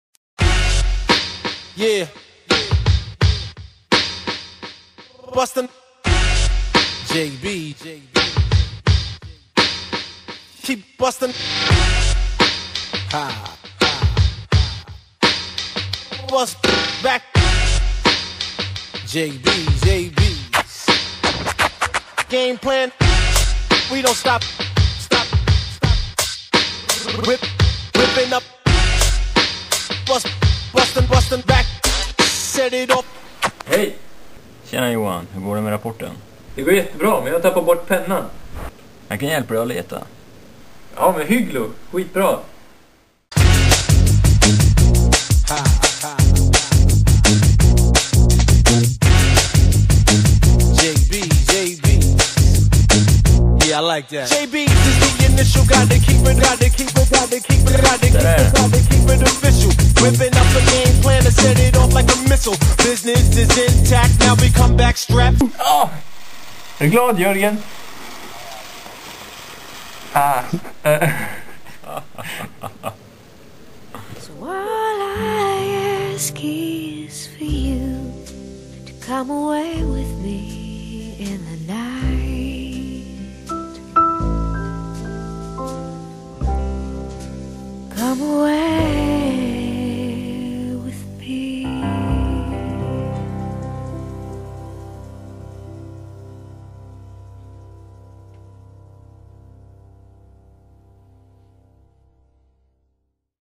Physical Avatar made of Lego
The idea of this university project was to distribute physical presence. The project group will develop an remote controlled robot that can represent a real person in any other place in the world.

The project group will build an fully functionable prototype robot with an remote controlled interface in a Lego (Lejos) computer. The robot may be controlled over an ordinary Internet connection. The avatar will be equipped with necessary gadgets as a Tablet PC, WLAN, IR, Web camera, microphone, speakers and Marratech communications software.